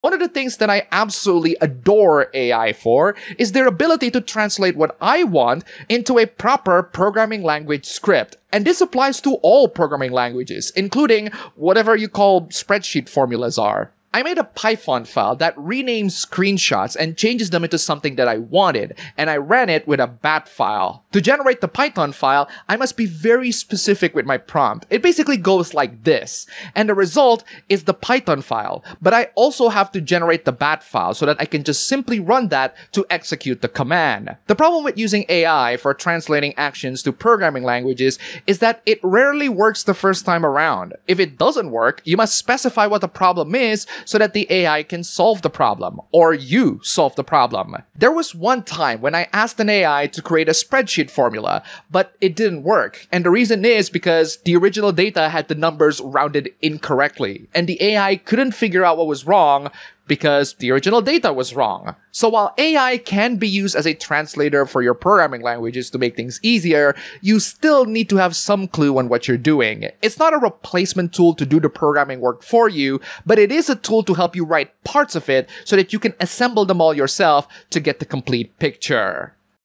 AI as Programming TRANSLATOR!
One of the things that I absolutely adore AI for is their ability to translate what I want into a proper programming language script, and this applies to all programming languages, including whatever you call spreadsheet formulas are.

I made a Python file that renames screenshots and changes them into something that I wanted, and I ran it with a .bat file. To generate the Python file, I must be very specific with my prompt. It basically goes like this, and the result is the Python file, but I also have to generate the .bat file so that I can just simply run that to execute the command.

The problem with using AI for translating actions to programming languages is that it rarely works the first time around. If it doesn't work, you must specify what the problem is so that the AI can solve the problem, or you solve the problem. There was one time when I asked an AI to create a spreadsheet formula, but it didn't work, and the reason is because the original data had the numbers rounded incorrectly, and the AI couldn't figure out what was wrong because the original data was wrong.

So, while AI can be used as a translator for your programming languages to make things easier, you still need to have some clue on what you're doing. It's not a replacement tool to do the programming work for you, but it is a tool to help you write parts of it so that you can assemble them all yourself to get the complete picture.